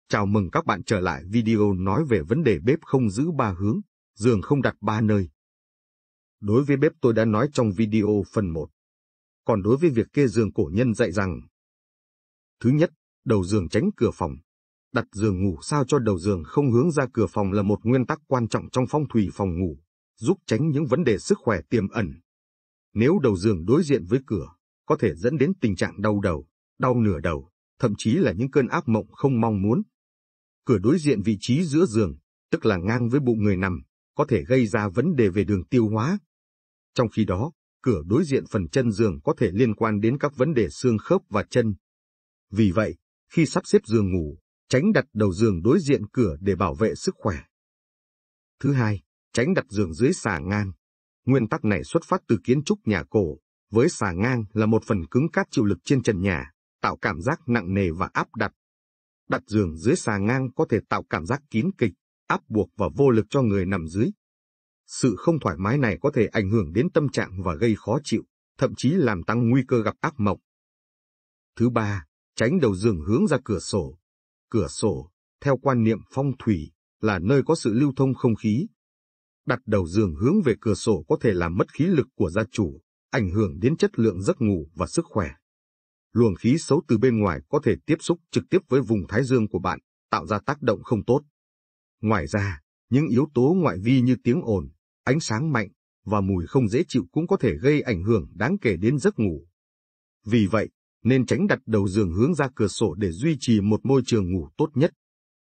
Bếp không giữ 3 hướng, giường không đặt 3 nơi - Phần 2
Thứ nhất: Đầu giường tránh cửa phòng. Đặt giường ngủ sao cho đầu giường không hướng ra cửa phòng là một nguyên tắc quan trọng trong phong thủy phòng ngủ, giúp tránh những vấn đề sức khỏe tiềm ẩn. Nếu đầu giường đối diện với cửa, có thể dẫn đến tình trạng đau đầu, đau nửa đầu, thậm chí là những cơn ác mộng không mong muốn. Cửa đối diện vị trí giữa giường (tức là ngang với bụng người nằm) có thể gây ra vấn đề về đường tiêu hóa. Trong khi đó, cửa đối diện phần chân giường có thể liên quan đến các vấn đề xương khớp và chân. Vì vậy, khi sắp xếp giường ngủ, tránh đặt đầu giường đối diện cửa để bảo vệ sức khỏe.
Thứ hai: Tránh đặt giường dưới xà ngang. Nguyên tắc này xuất phát từ kiến trúc nhà cổ, với xà ngang là một phần cứng cáp chịu lực trên trần nhà, tạo cảm giác nặng nề và áp đặt. Đặt giường dưới xà ngang có thể tạo cảm giác kín kịch, áp buộc và vô lực cho người nằm dưới. Sự không thoải mái này có thể ảnh hưởng đến tâm trạng và gây khó chịu, thậm chí làm tăng nguy cơ gặp ác mộng.
Thứ ba: Tránh đầu giường hướng ra cửa sổ. Cửa sổ, theo quan niệm phong thủy, là nơi có sự lưu thông không khí. Đặt đầu giường hướng về cửa sổ có thể làm mất khí lực của gia chủ, ảnh hưởng đến chất lượng giấc ngủ và sức khỏe. Luồng khí xấu từ bên ngoài có thể tiếp xúc trực tiếp với vùng thái dương của bạn, tạo ra tác động không tốt. Ngoài ra, những yếu tố ngoại vi như tiếng ồn, ánh sáng mạnh, và mùi không dễ chịu cũng có thể gây ảnh hưởng đáng kể đến giấc ngủ. Vì vậy, nên tránh đặt đầu giường hướng ra cửa sổ để duy trì một môi trường ngủ tốt nhất.
Bếpkhônggiữ3hướng,giườngkhôngđặt3nơi#phongthuy